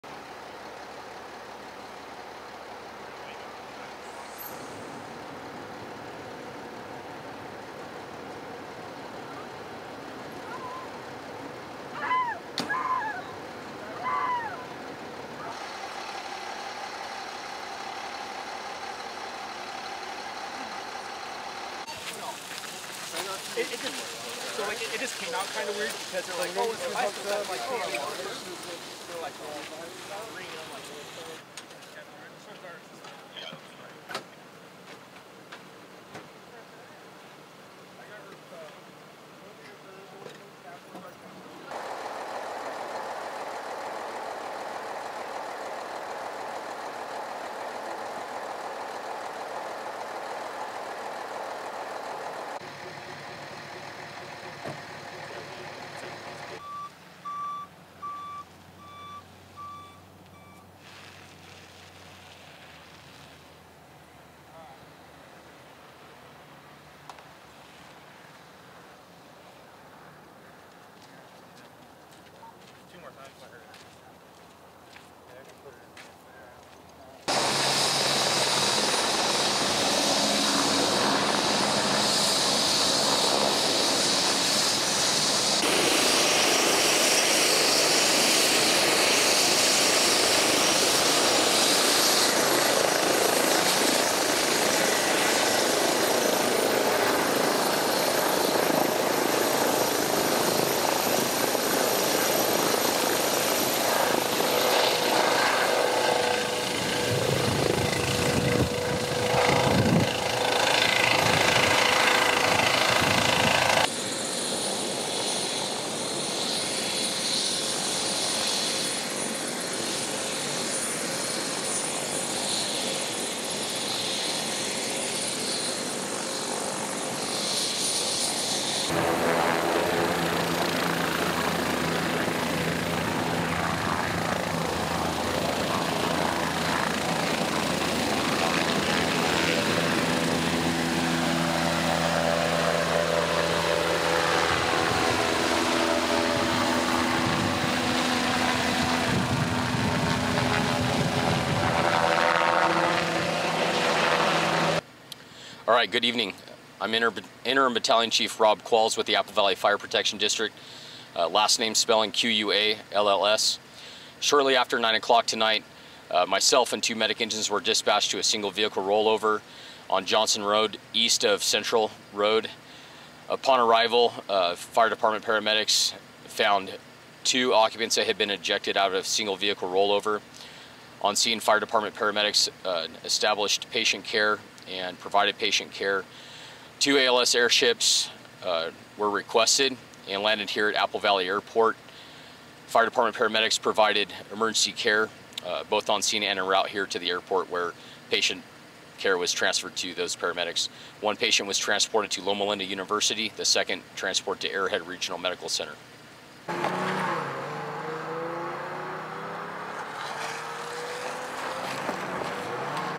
Two Women Ejected in Solo Roll Over Crash | Apple Valley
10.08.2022 | 10:07 PM | APPLE VALLEY – Two women were ejected from a solo vehicle roll over.

Vehicle rolled on the dirt portion of Johnson Road east of Central Road.

Two Air ambulances were ordered to land a Apple Valley Airport.

Victims were transported by ground ambulance to Apple Valley Airport.

One Victim was taken to Arrowhead Regional Medical Center.

One Victim was taken to Loma Linda University Hospital.